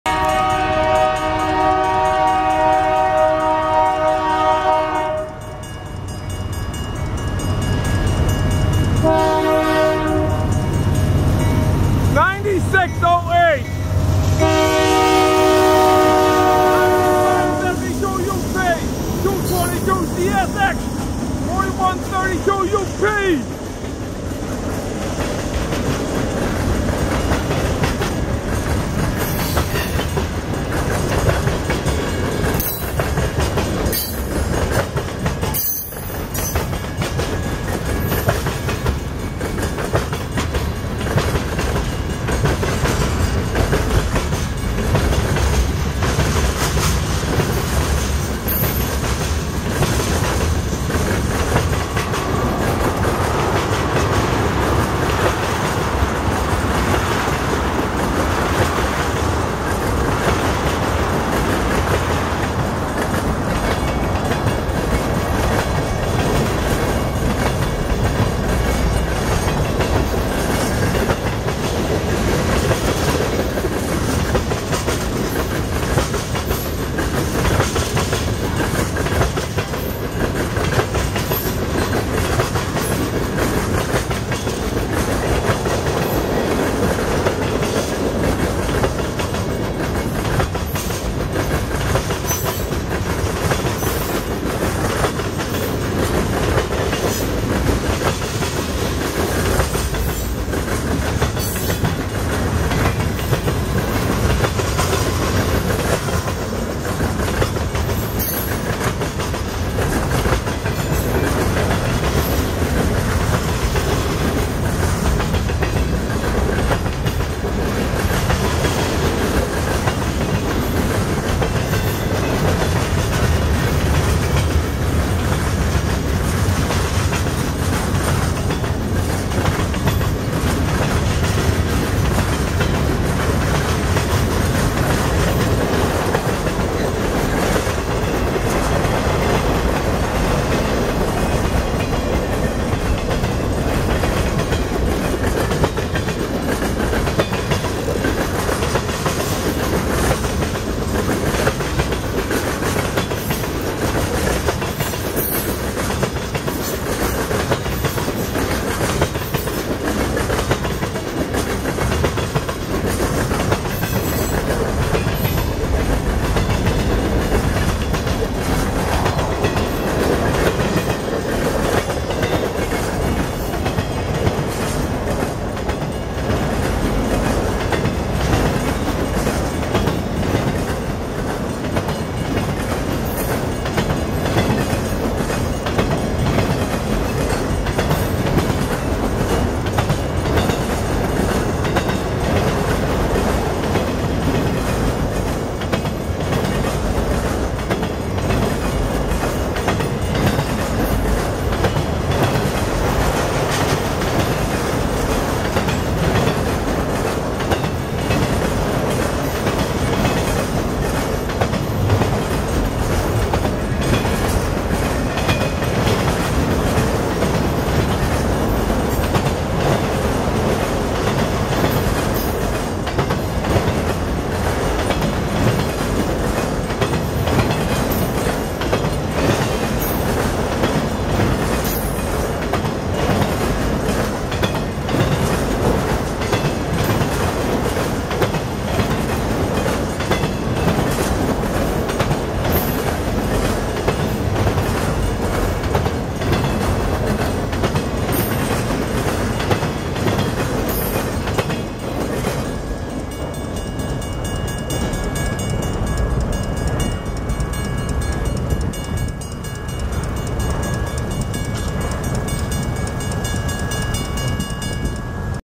Southbound CP 9608, UP 9572 and CSX 222 and UP 4132 on the CPRS
This is very interesting and when and there are two UP's and a CSX on the Canadian Pacific Rail System and this is located on IL Rte. 176 and just east of I-94.